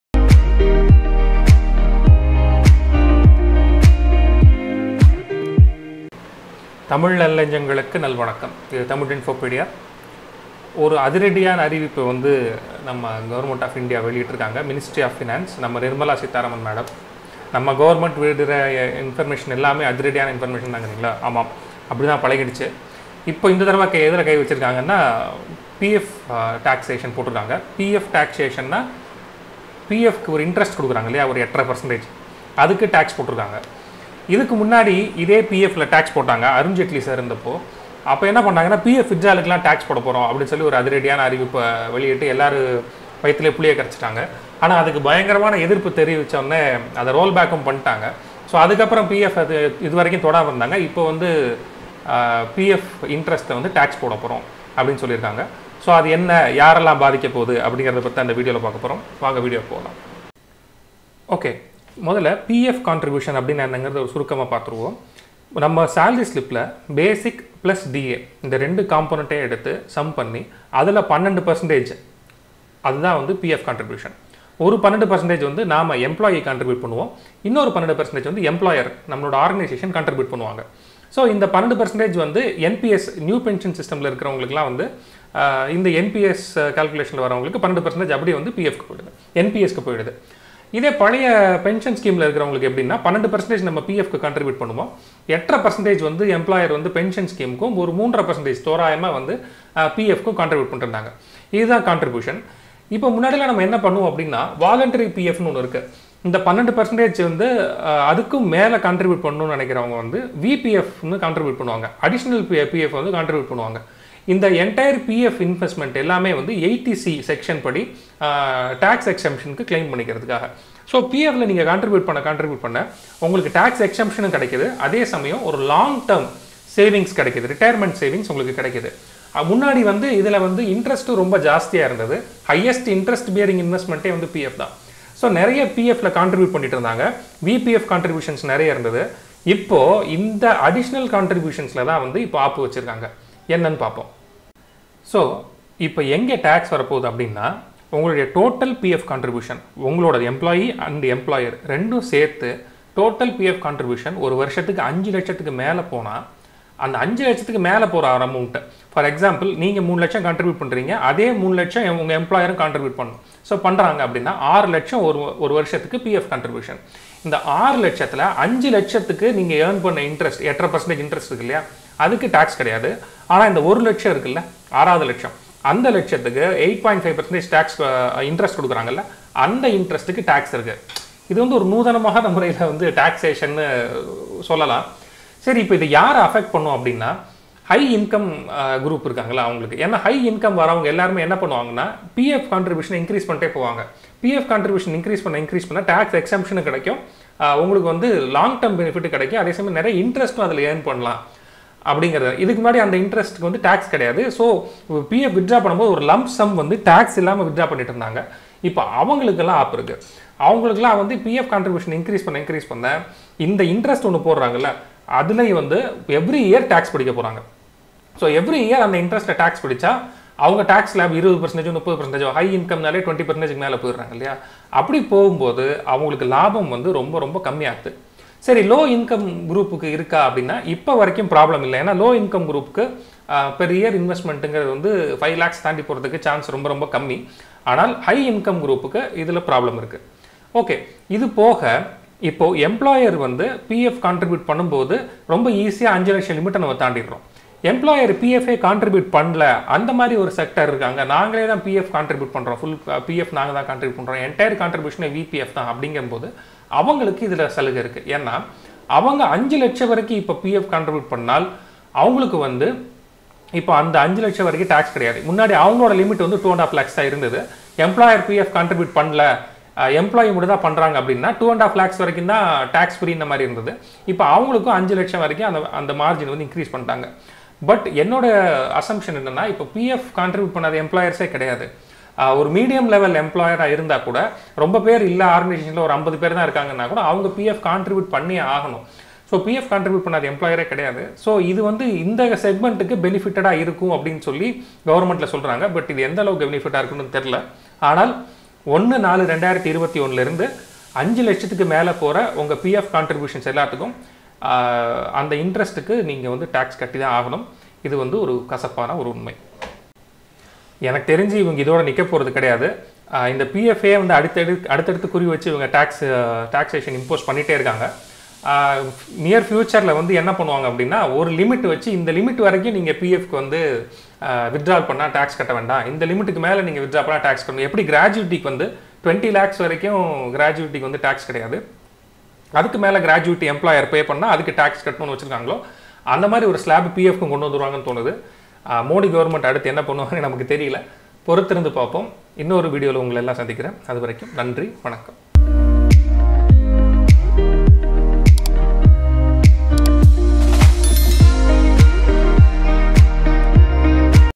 Tax on PF interest in Budget 2021 - Tamil
This video discusses about tax on pf interest in budget 2021. We have explained it in Tamil. This is the second attempt of Central Govt. to tax PF components. In 2016, they failed in implementing tax of PF withdrawl. Now they have implemented tax of PF where contribution is more than Rs. 5.00 lakhs.